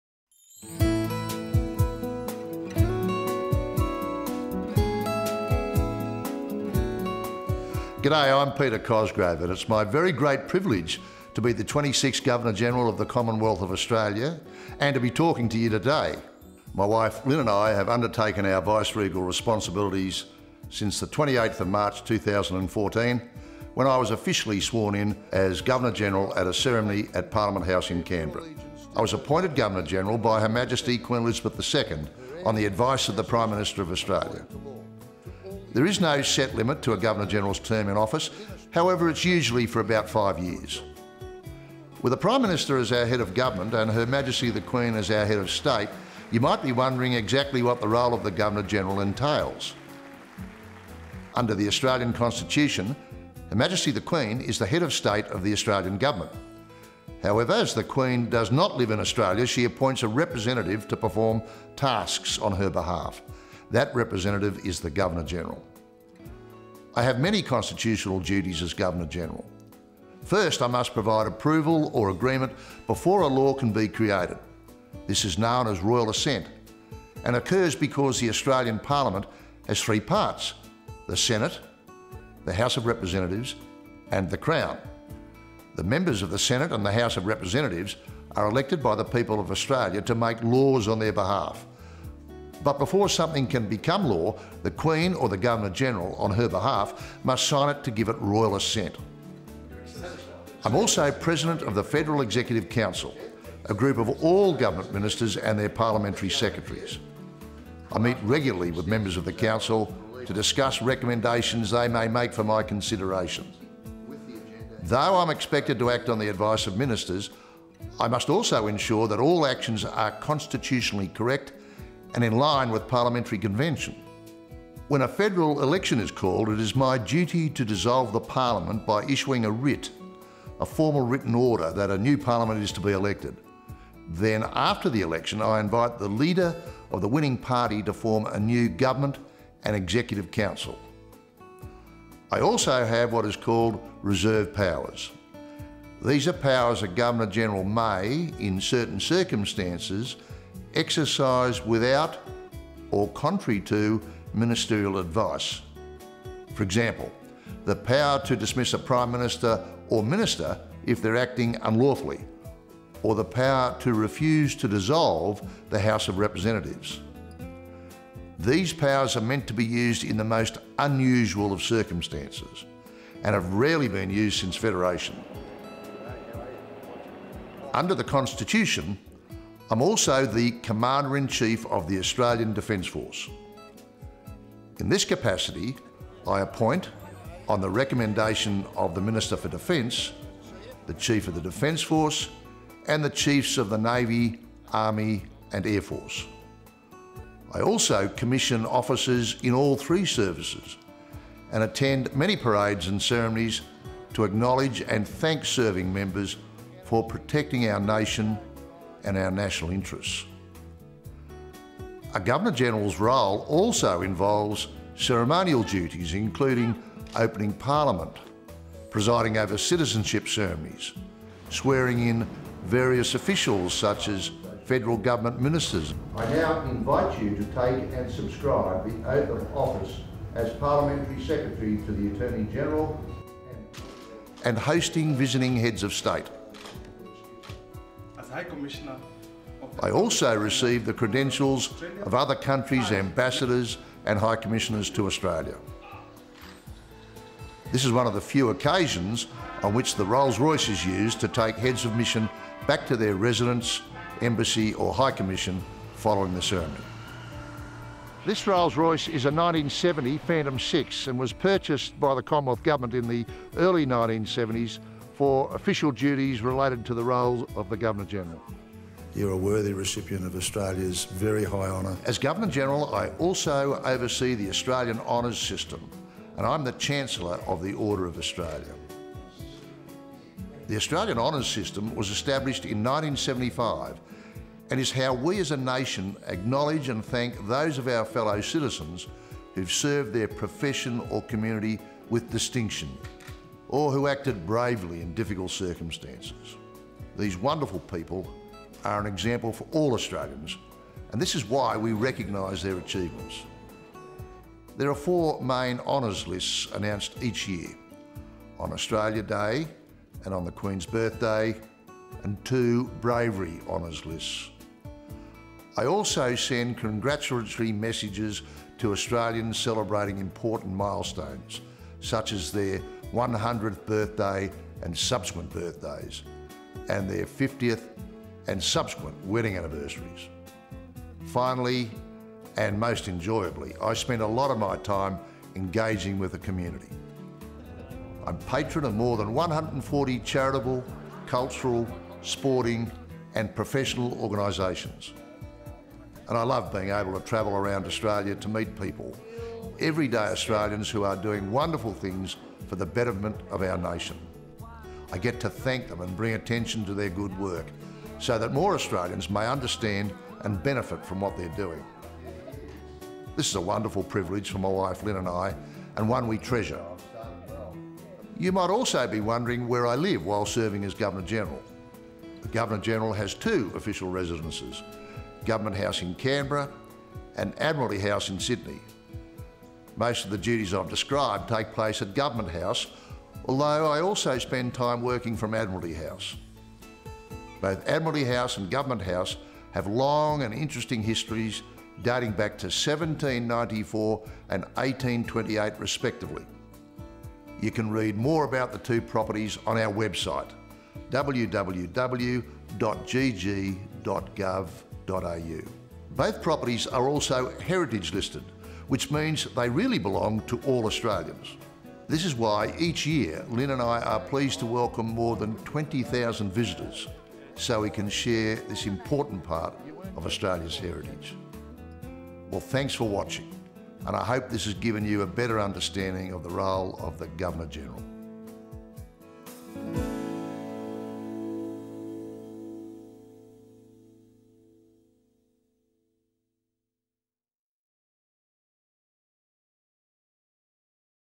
2015 GG Sir Peter Cosgrove web video
Beyond the Box Pictures was commissioned to create a web video that explained the Role of the Governor General. This was an educational video for school children and the wider community. The video was created from material shot by Beyond The Box Pictures and file vision. 2015